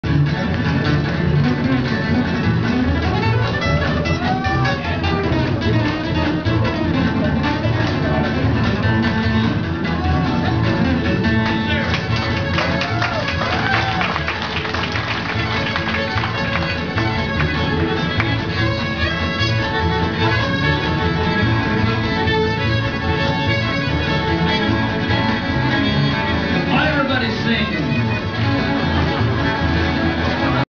Wilford Brimley on guitar
Wilford Brimley kickin' butt playing lead guitar in a cowboy band!